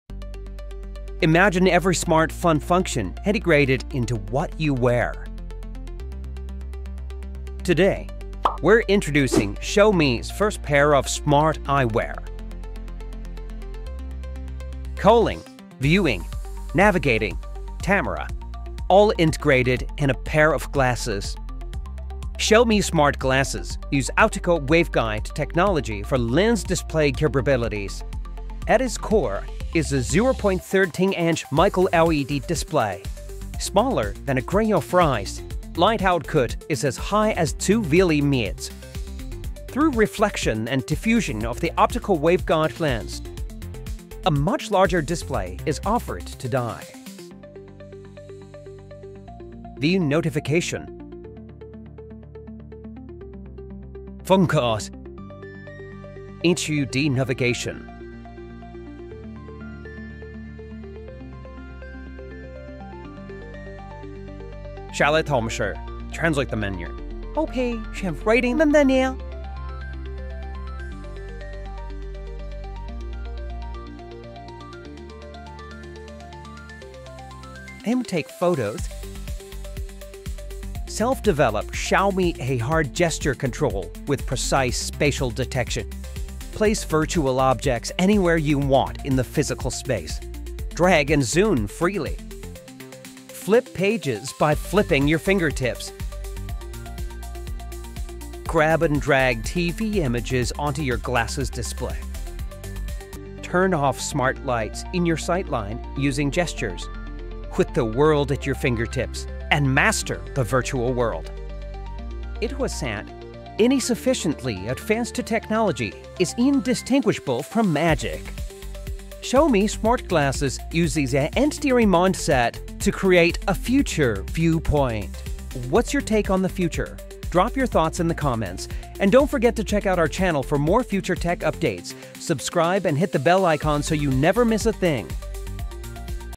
Xiaomi’s New AI Glasses Are Smarter Than You Think 1
Xiaomi just unveiled its most advanced wearable yet — the Xiaomi AI Glasses, and they're packed with cutting-edge features like:

Are these glasses better than Meta’s Ray-Ban Smart Glasses?